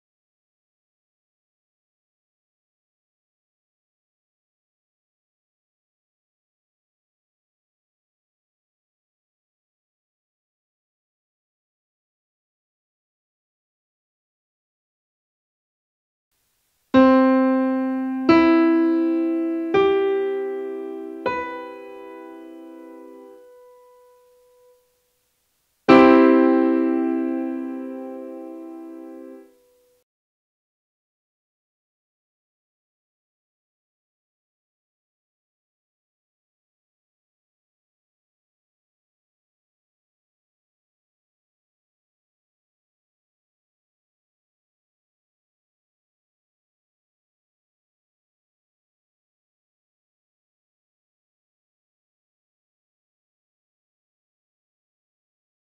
B# Major 7 (B#maj7) Chord: How To Play It On Piano!
Are you looking for how to play the B# Major 7 (B#maj7) Chord on the Piano?

Hello!

In this piano tutorial video, I will show you how to play the B# Major 7 (B#maj7) Chord on the piano. This lesson is designed for beginners or those who want to brush up on their knowledge of the B# Major 7 (B#maj7) Chord.

The A# Major 7 (A#maj7) Chord uses the following notes: B# Dx Fx Ax

In music, a major seventh chord is a seventh chord in which the third is a major third above the root and the seventh is a major seventh above the root. The major seventh chord, sometimes also called a Delta chord, can be written as maj7, M7, Δ, ⑦, etc. The "7" doesn't have to be superscripted, but if it is, then any alterations, added tones, or omissions are usually also superscripted. For example, the major seventh chord built on C, commonly written as Cmaj7, has pitches C–E–G–B:

Chords are an important aspect of piano playing, and everyone goes through the process of learning triads by heart.

When practicing always start by playing slowly! Once you get more assurance you can increase speed. Practice frequently (daily) and keep your sessions short, especially if you are a beginner.